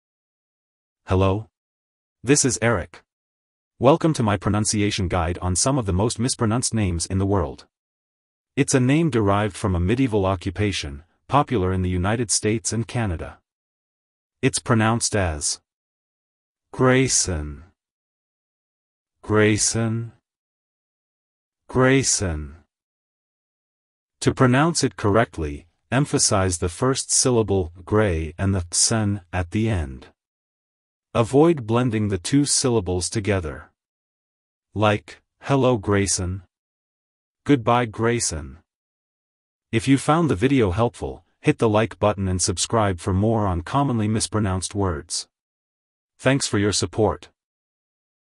How to Pronounce Name Graysen (Correctly!)
Welcome! Here you will hear how to pronounce Graysen correctly.
It's a name derived from a medieval occupation, popular in the United States and Canada.
To pronounce it correctly, emphasize the first syllable 'Gray' and the 'sen' at the end. Avoid blending the two syllables together.

08a0